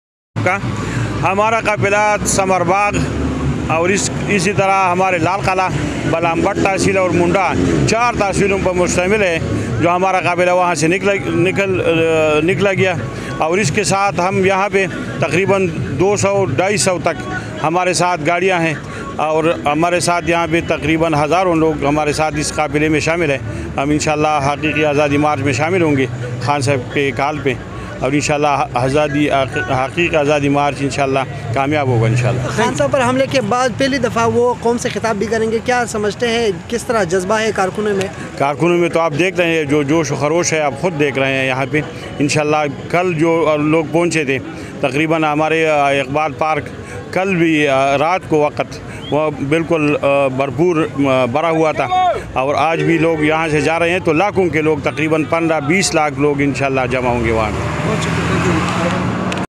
MNA Muhamad Bashir k qeyadat mai qafila Islamabad Kelly rawana hogiya
Get ready to witness a momentous event as MNA Muhamad Bashir, a dynamic and influential political leader, leads a spirited caravan departure from Kelly to Islamabad. This captivating video takes you on a thrilling journey as the convoy, filled with passionate supporters, embarks on a mission to advocate for the rights and aspirations of the people.